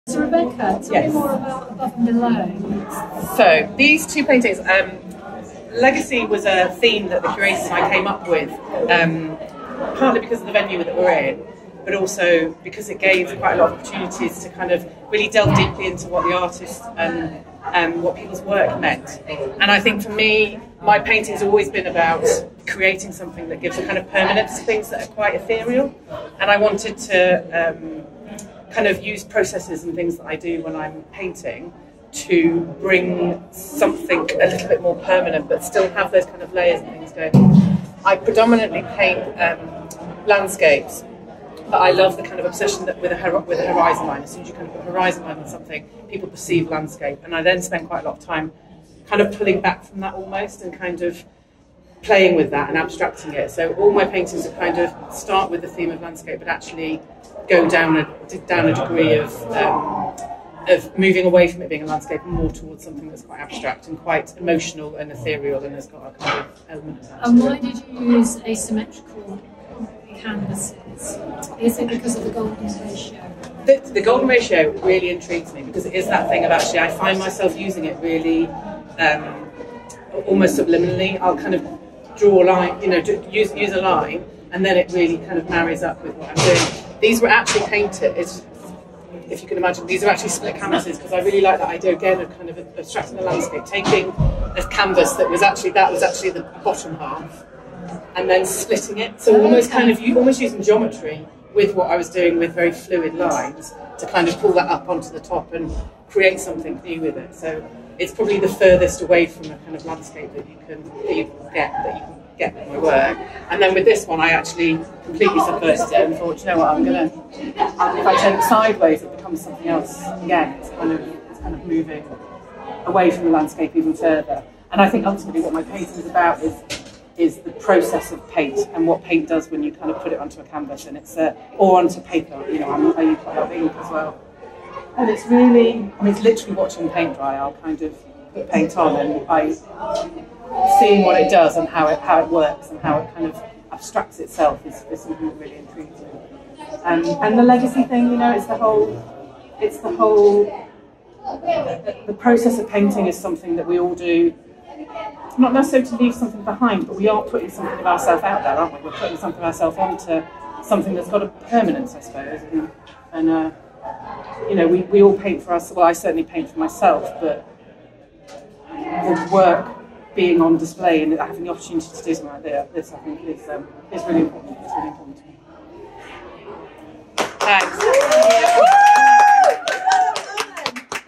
How to leave your legacy with art, artist talk Rebecca Tucker, Legacy, an ArtCan Exhibition London.
Legacy Artists Talk - 6th September 2022  (extract from the evening)

Curator Rebecca Tucker shares her thoughts about her piece on show.

Legacy at the Crypt Gallery, London
Curated by: Rebecca Tucker, Jill Desborough and Lee Eveson

This exhibition exploring the theme of 'Legacy' was shown in the wonderfully
atmospheric Crypt Gallery under St. Pancras Church, Euston Road in London.

To know more about our International Artist Community ARTCAN and for art purchase enquiries, please visit:

Video editing: Candice Wu